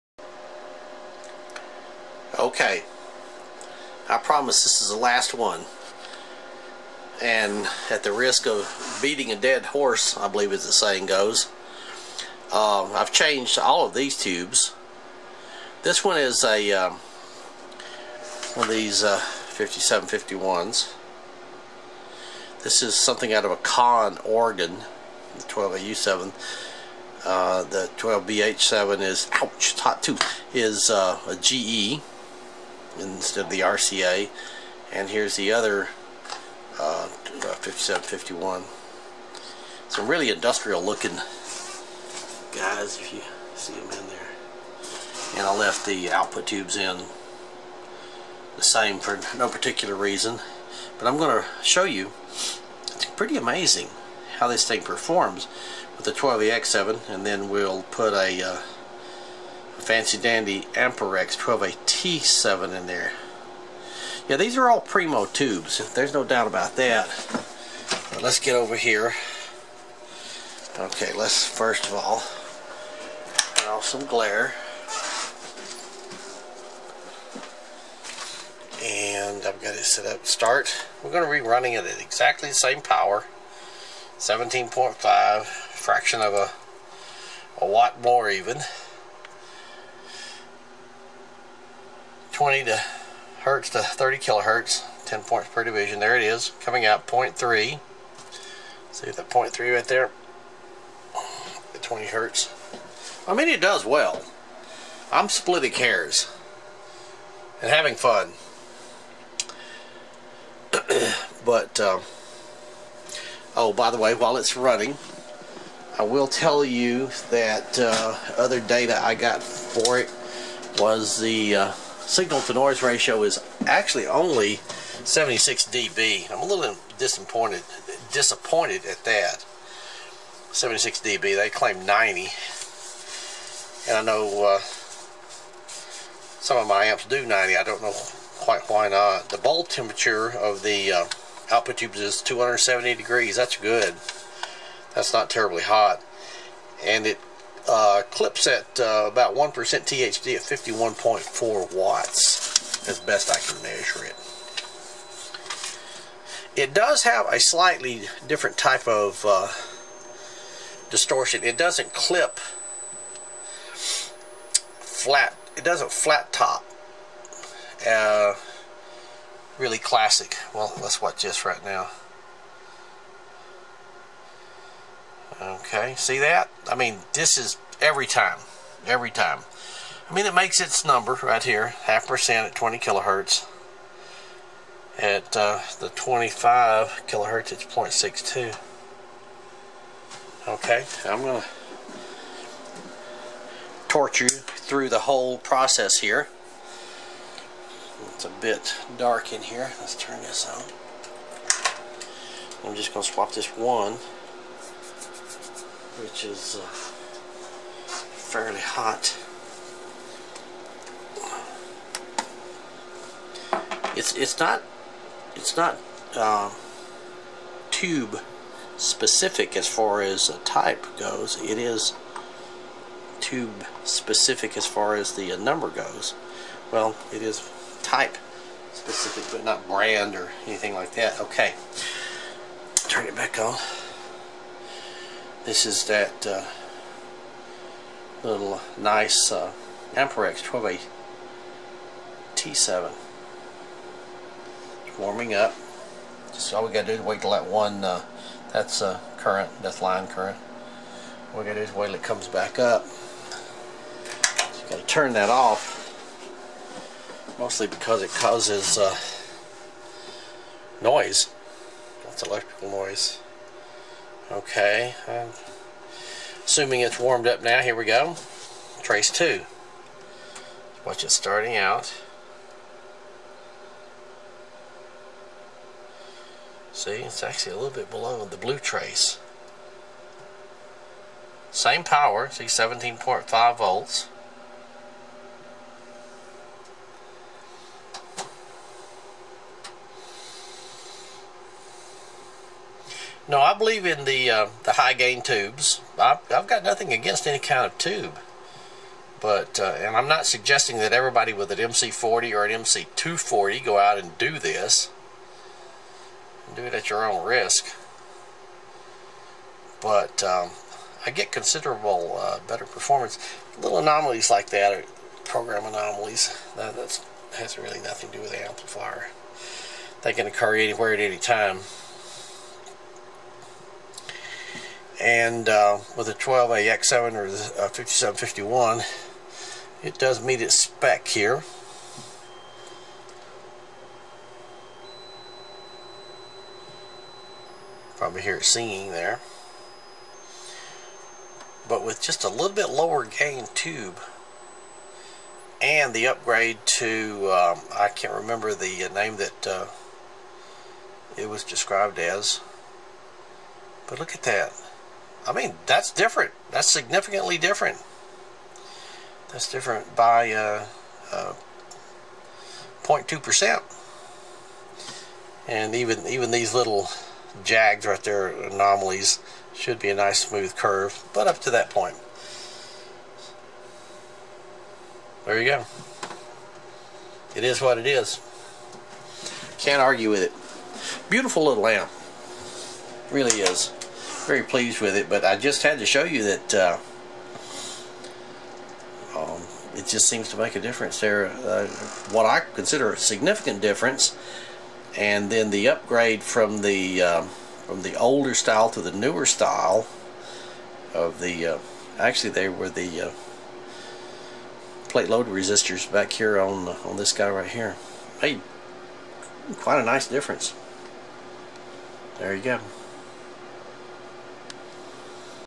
Comparing 12AX7 vs 12AT7 As The Driver In The McIntosh MC40
Last analysis of this amp - so far, I have seen no anomalies in parasitic or instabilities that the substitution of 12AX7 for 12AT7 has caused in the Mac  MC 40.  However, you cannot substitute a 12AU7 in place of the 12AX7 or 12AT7.